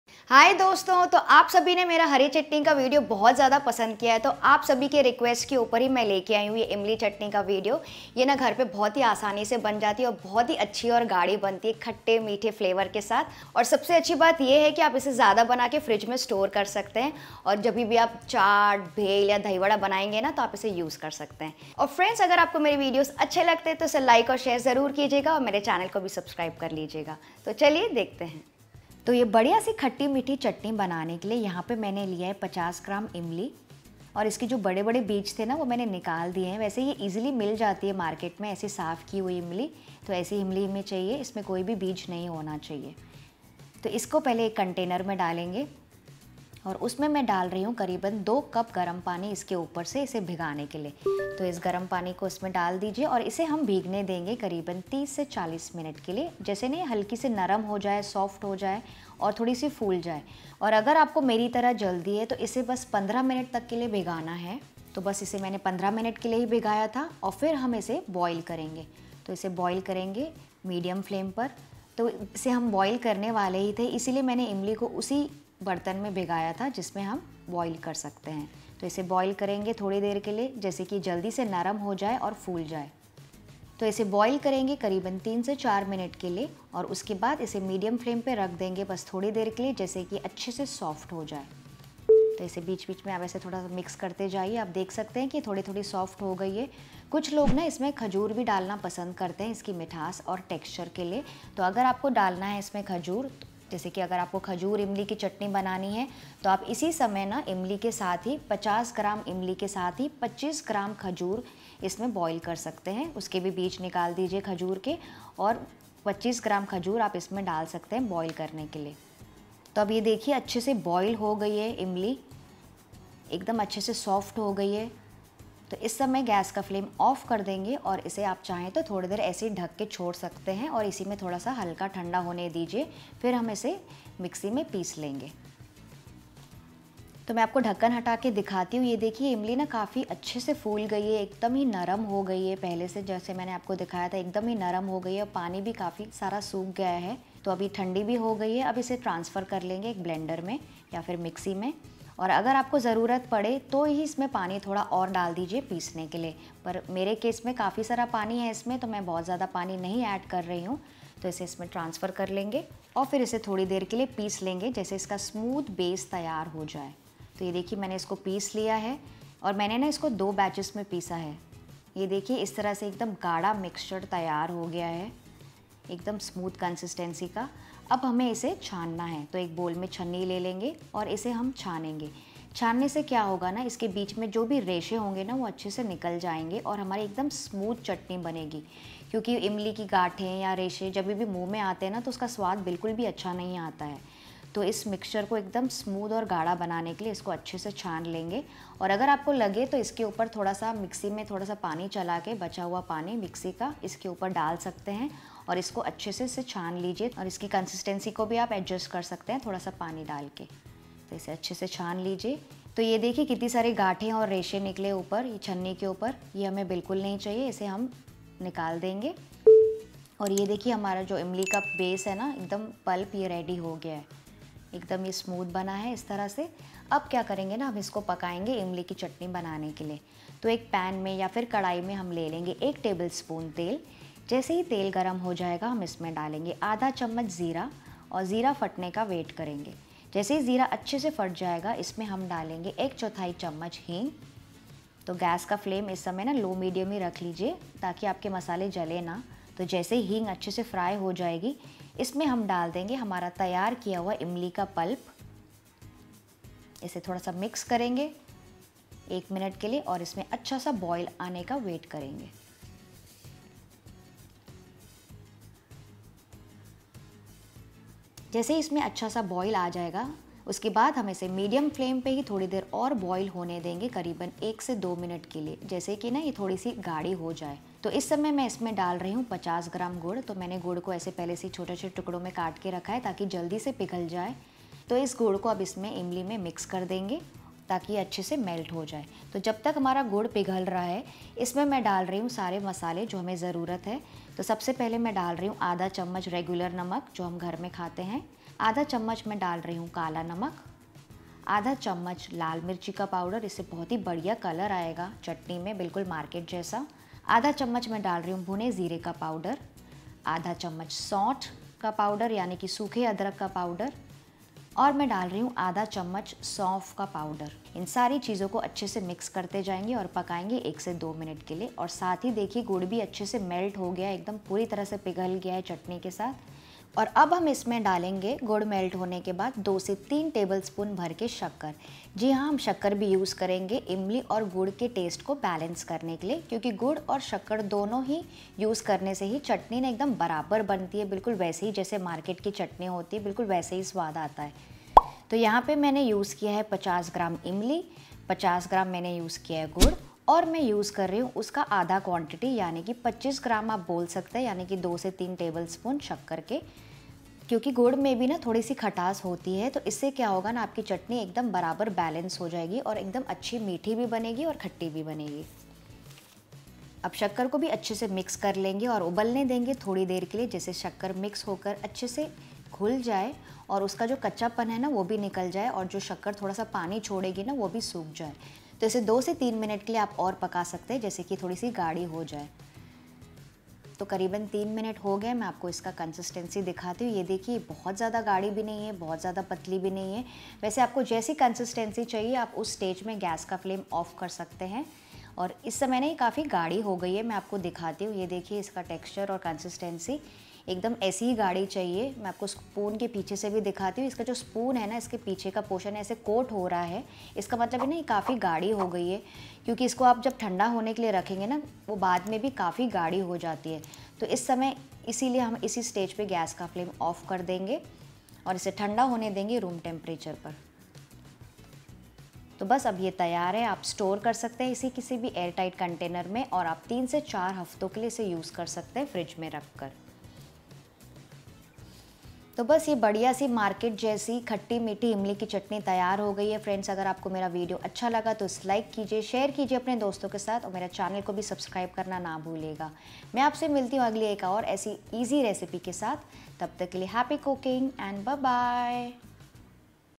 इमली की खट्टी मीठी चटनी बनाने का आसान तरीका | Imli Ki Chatni Recipe | Street Style Chutney For Chaat
Imli Ki Chutney | Imli Ki Khatti Meethi Chutney | Sweet Tamarind Chutney street style banane ka asaan tarkia is recipe video main dekhe aaj. Dahi vade ya chaat ke saath yeah recipe tasty rehti hain. Dosa ke saath bhi aap ise kha sakte hain. Video acha laga to jaroor like aur share karein apne friends aur family ke saath.

INGREDIENTS:
50 gms imli/tamarind
1 tbsp oil
1/2 tsp jeera
1/4 tsp heeng
50 gms gud/jaggery
1/2 tsp salt
1/2 tsp kala namak
1/2 tsp red chilly powder
1/2 tsp jeera powder
1/2 tsp soth/adrak powder
2-3 tbsp sugar

▬▬▬▬▬ Best Kitchen Products I Recommend▬▬▬▬▬